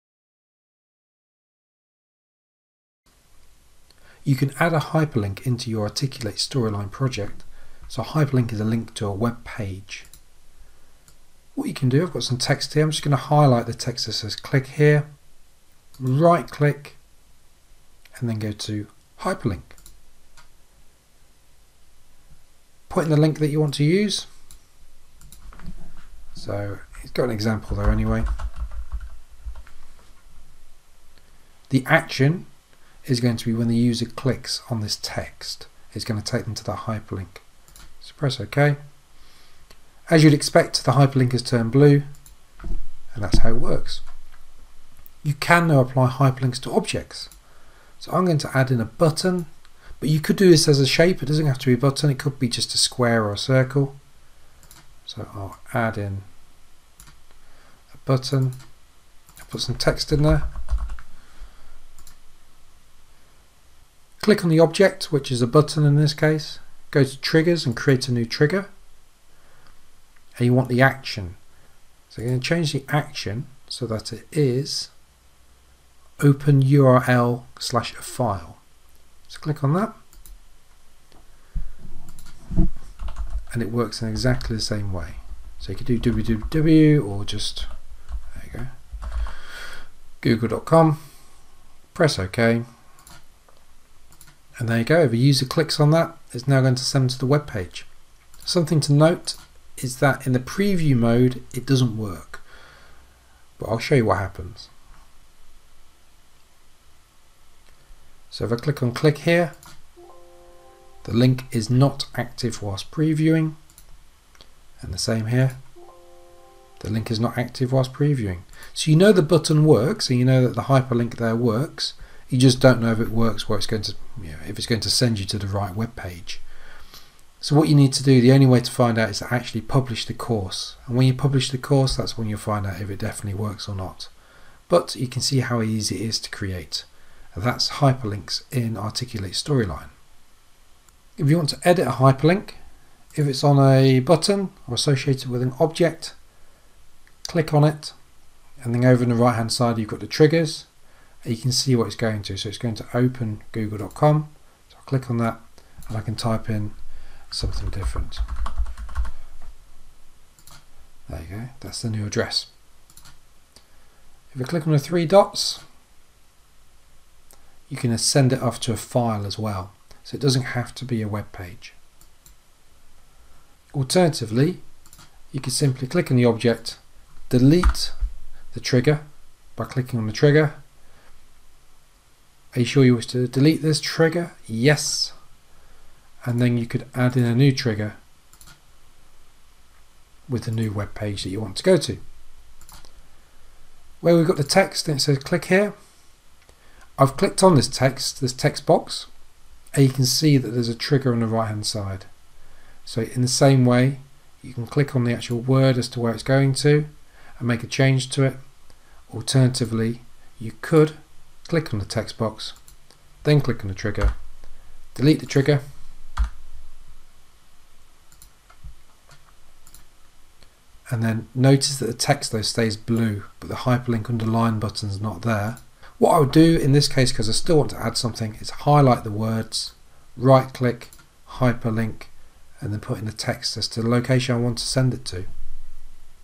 Inserting and Editing HYPERLINKS in Articulate Storyline
Learn how to use, insert and edit hyperlinks in Articulate Storyline. This could be a hyperlink to a website, intranet, a document stored on a server. Whatever you want really.